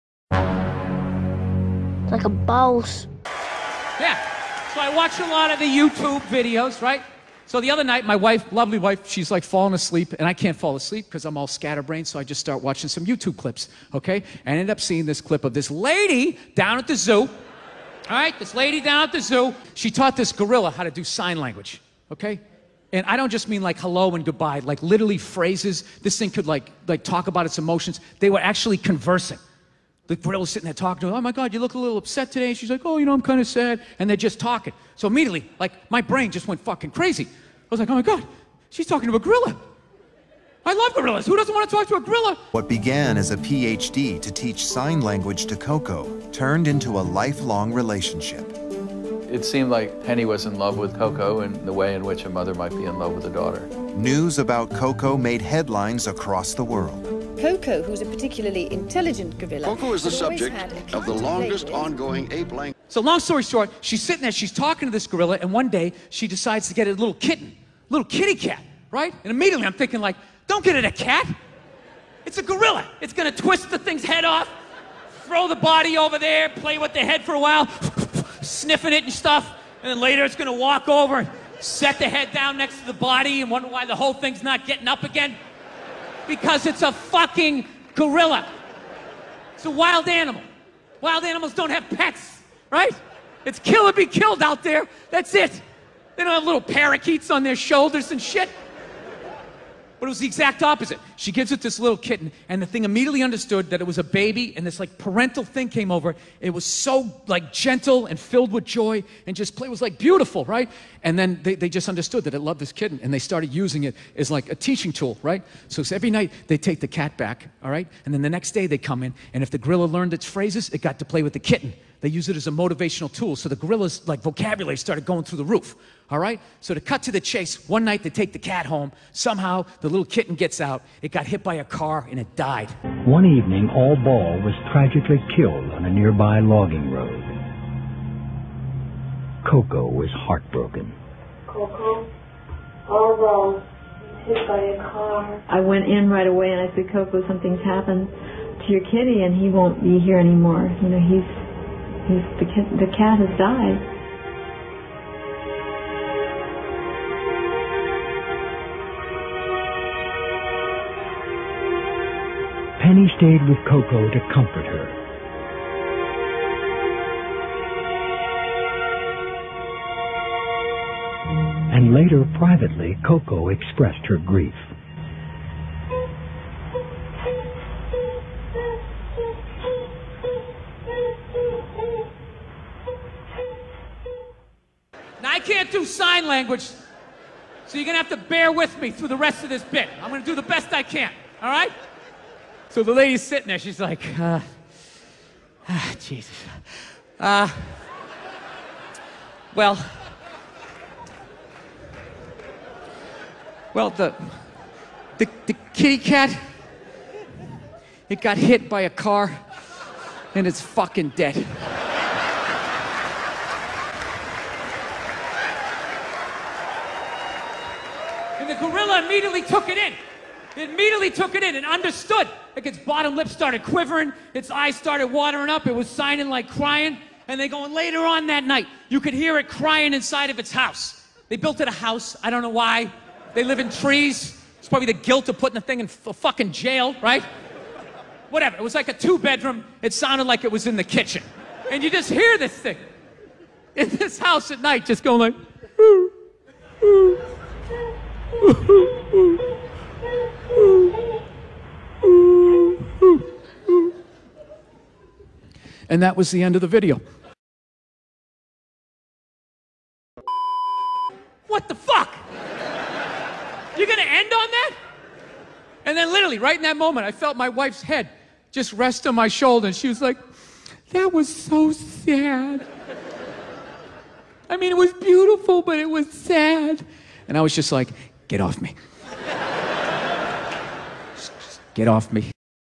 Bill Burr - Koko the Gorilla
Bill Burr talks about Koko the Gorilla.

Hanabiko "Koko" was a female western lowland gorilla known for having learned many hand signs from a modified version of American Sign Language. Koko was born at the San Francisco Zoo and lived most of her life in Woodside, California, at The Gorilla Foundation's preserve in the Santa Cruz Mountains.